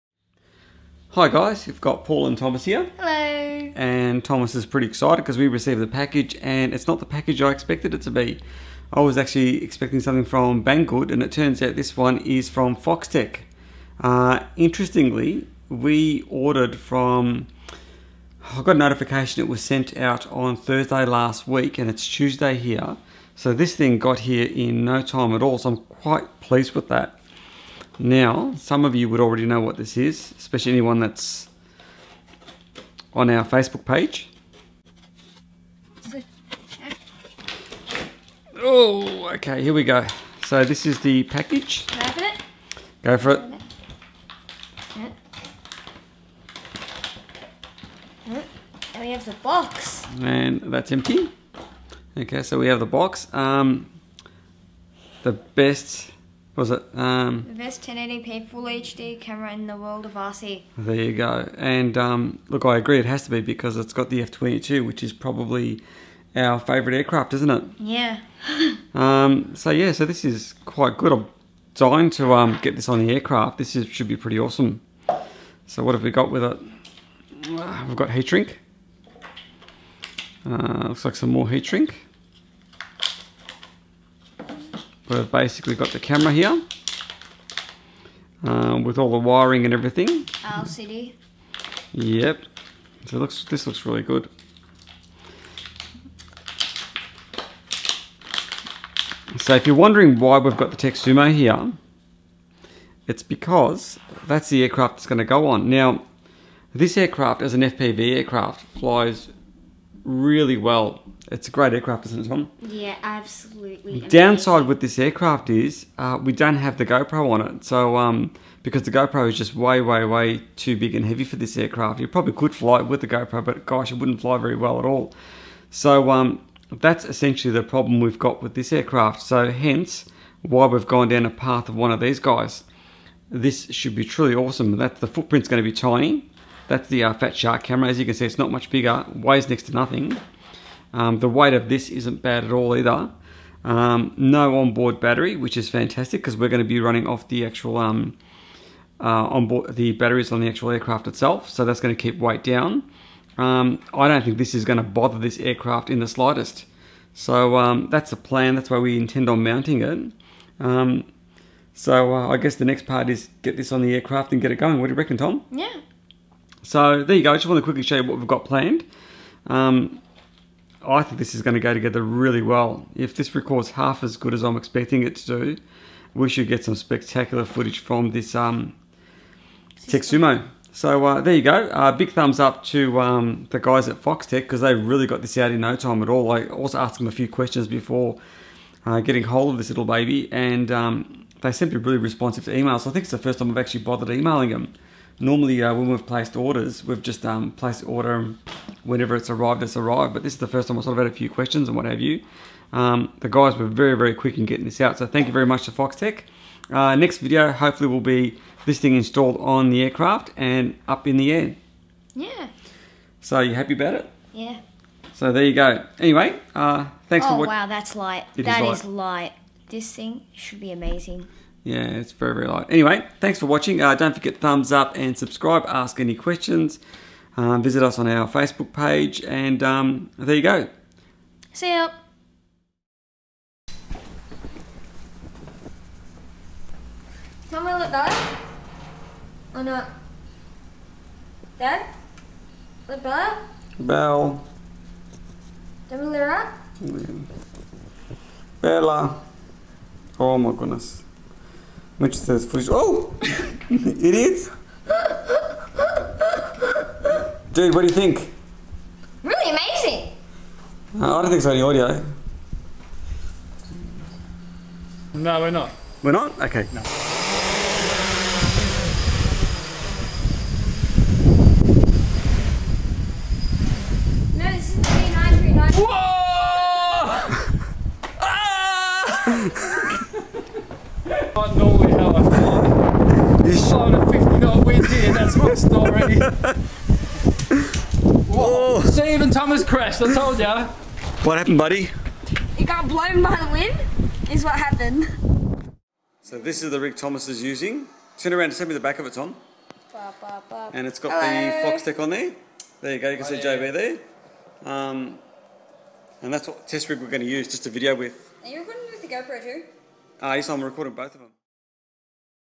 HoryzonHD Full HD 1080p FPV Camera V3 - UnBoxing & Sample Footage
This video will cover HoryzonHD Full HD 1080p FPV Camera V3 Unboxing, thoughts and sample footage. I must say I was very pleasantly surprised with the vision from this camera. Ground test was successful and its just a matter of getting it on the aircraft now!

Will also do a comparison video between it and the Hero3 Soon.

Looking forwards to getting it on Thomas' aircraft next :D

Thanks for viewing his :)